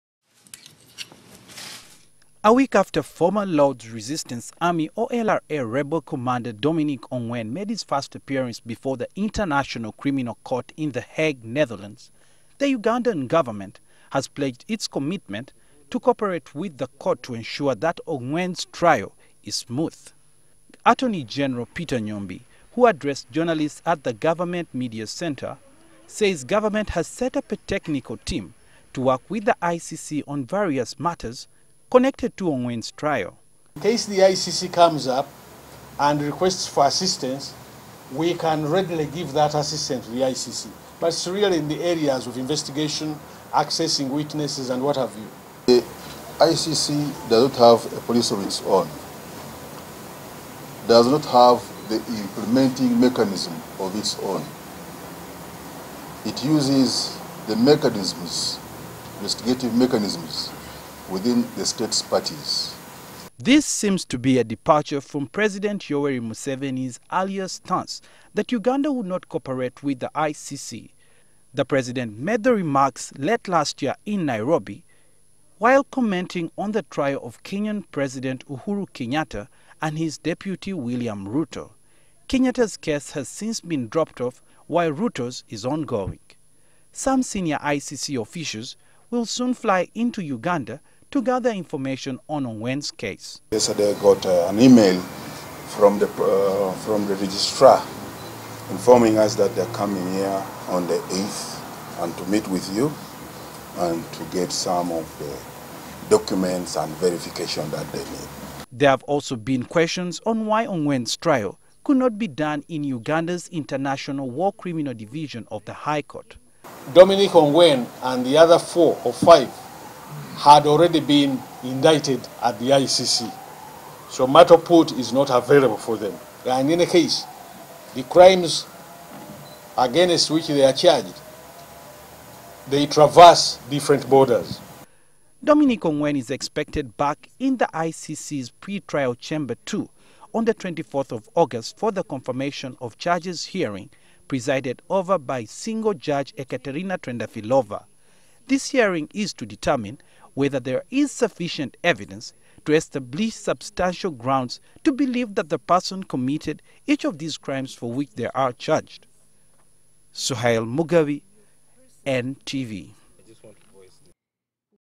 ICC Registrar to visit Uganda over Ongwen case
The International Criminal Court at the  Hague  has  formally written to the Ugandan Government to verify documents.This  days after  LRA’s commander Dominic Ongwen appeared before the war crimes court for a confirmation  hearing.The  court’s registrar is also set to visit the country.The Attorney General Peter Nyombi also confirms that  the  technical committee has set up a technical committee to respond to any of the Court’s requests.